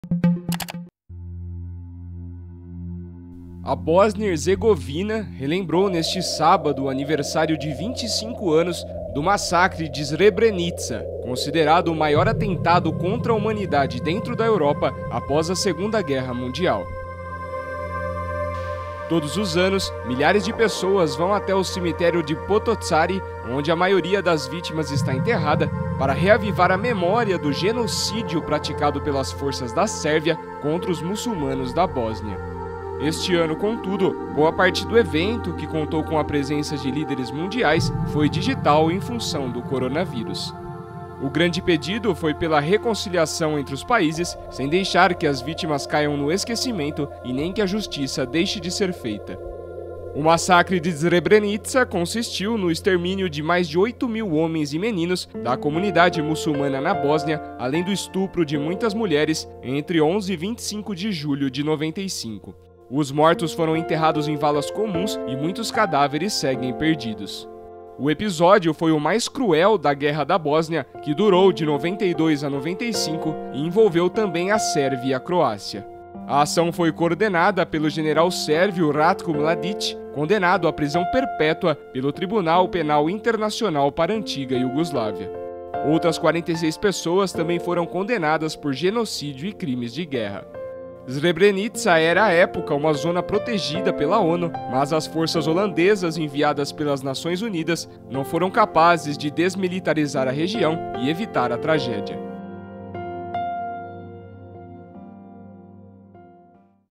Massacre da Bósnia completa 25 anos
Srebrenica, 11 jul (EFE/EPA).- (Imagens: Fehim Nedir) Bósnia-Herzegovina relembrou neste sábado o aniversário de 25 anos do massacre de Srebrenica, considerado o maior atentado contra a humanidade dentro da Europa após a Segunda Guerra Mundial.